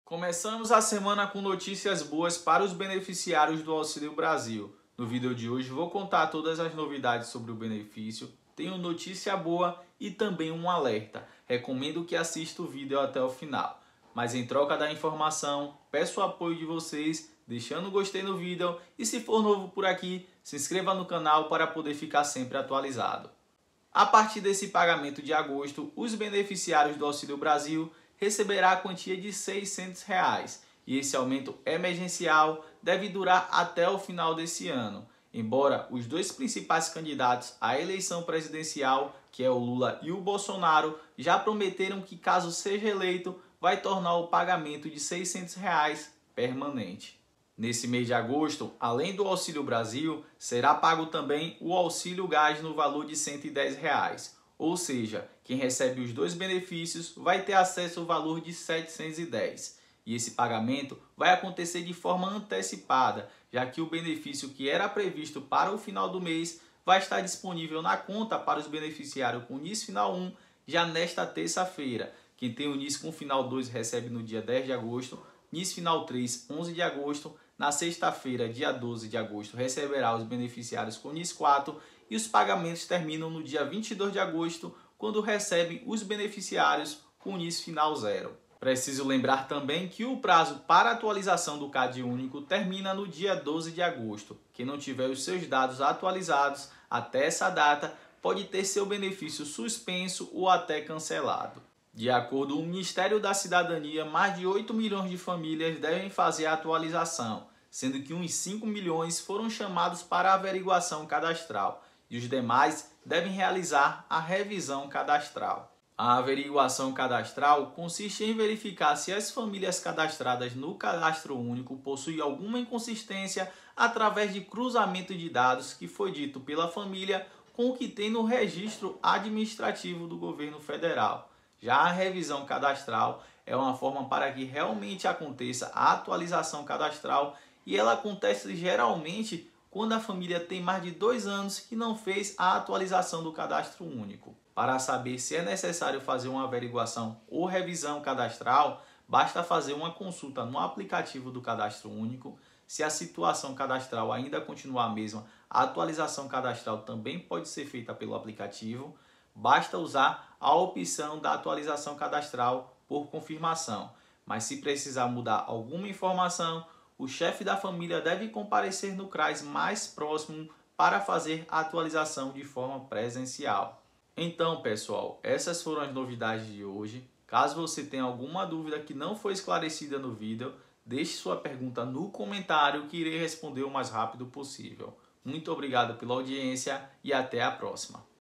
Vai ser pago 600 do Auxílio Brasil
A partir desse pagamento de agosto os beneficiários do auxilio brasil recebera a quantia de 600 reais e esse aumento emergencial deve durar até o final desse ano. Embora os dois principais candidatos a eleição presidencial que é o lula e o Bolsonaro, já prometerem que caso seja eleito vai tornar o pagamento de 600 reais permanente.